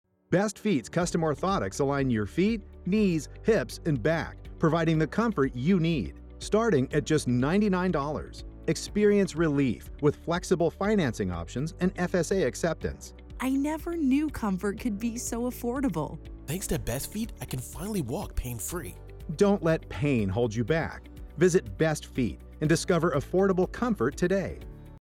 Affordable Custom Orthotics for Pain-Free Living | Best Feet
Discover how Best Feet's custom orthotics can provide the support and comfort you need for a pain-free life. With prices starting at just $99 and flexible financing options, including FSA acceptance, relief is more affordable than ever. Hear from our satisfied customers who have transformed their lives with Best Feet's orthotics. Don't let pain hold you back - visit Best Feet today and experience the difference for yourself.